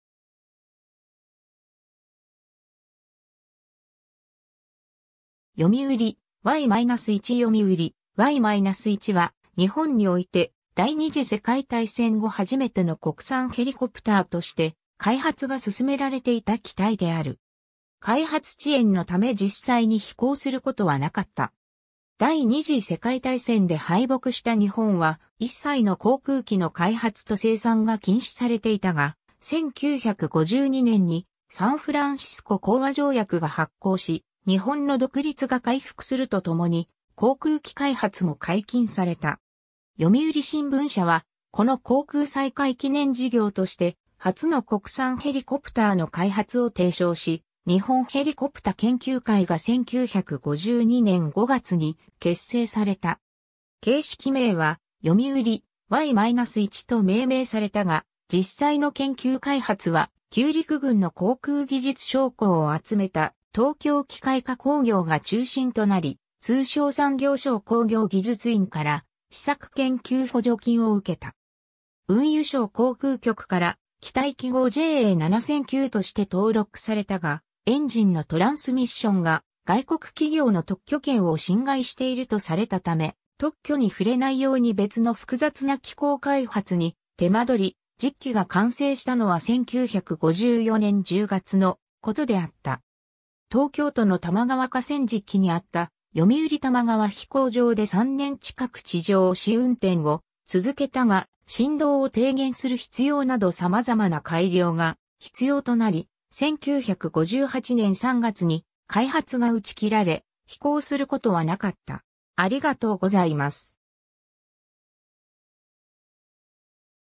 読売 Y-1
読売 Y-1 読売 Y-1（よみうり - ）は、日本において第二次世界大戦後初めての国産ヘリコプターとして開発が進められていた機体である。
開発遅延のため実際に飛行することは無かった。
 第二次世界大戦で敗北した日本は一切の航空機の開発と生産が禁止されていたが、1952年（昭和27年）にサンフランシスコ講和条約が発効し、日本の独立が回復するとともに、航空機開発も解禁された。
 読売新聞社は、この航空再開記念事業として初の国産ヘリコプターの開発を提唱し、日本ヘリコプタ研究会が1952年5月に結成された。
形式名は「読売 Y-1」と命名されたが、実際の研究開発は旧陸軍の航空技術将校を集めた東京機械化工業が中心となり、通商産業省（現経済産業省）工業技術院から試作研究補助金を受けた。
 運輸省航空局から機体記号JA7009として登録されたが、エンジンのトランスミッションが外国企業の特許権を侵害しているとされたため、特許に触れないように別の複雑な機構開発に手間取り、実機が完成したのは1954年（昭和29年）10月のことであった。
東京都の多摩川河川敷にあった読売玉川飛行場で3年近く地上試運転をつづけたが、振動を低減する必要など様々な改良が必要となり、1958年（昭和33年）3月に開発が打ち切られ、飛行することは無かった。